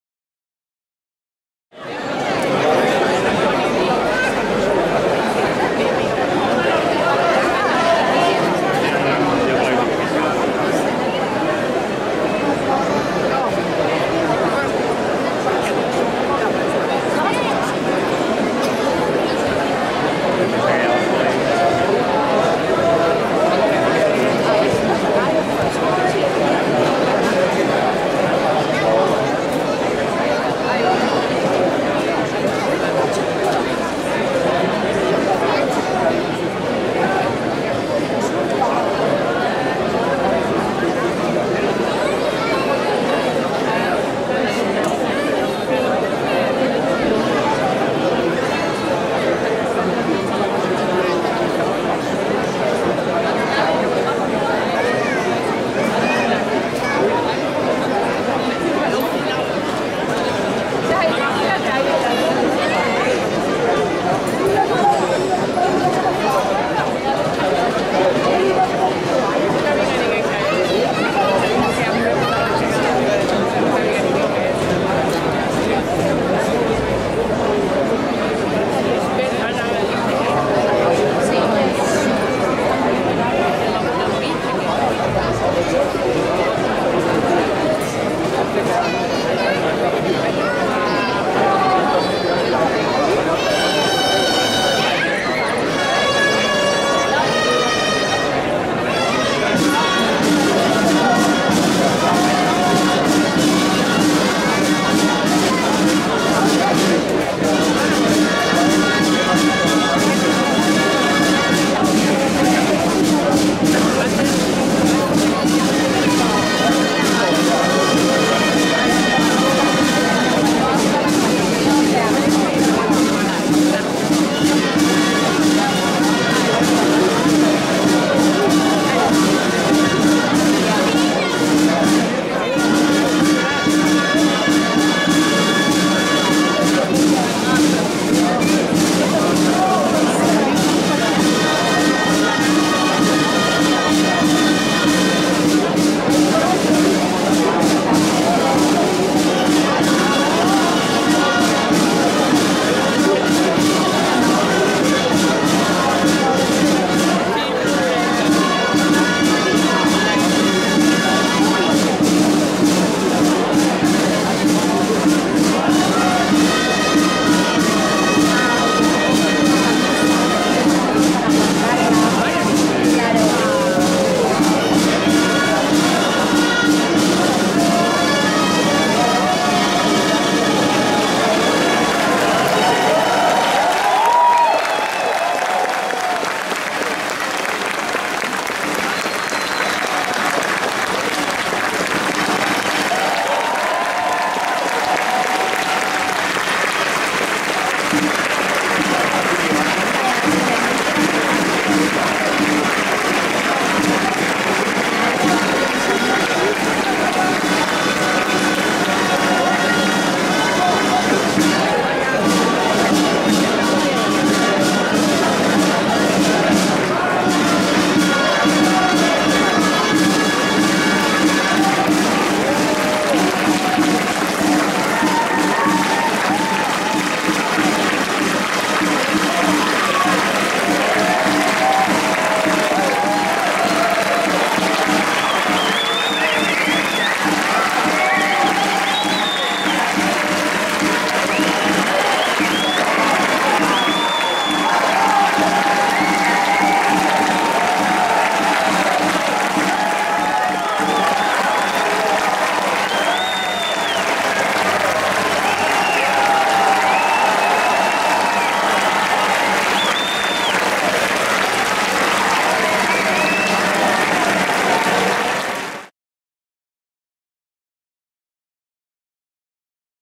7d8 Camp d’en Grassot 2024
7 de 8 descarregat a la Diada de la Festa Major del Camp d’en Grassot celebrada al carrer d’en Grassot, el diumenge 30 de juny de 2024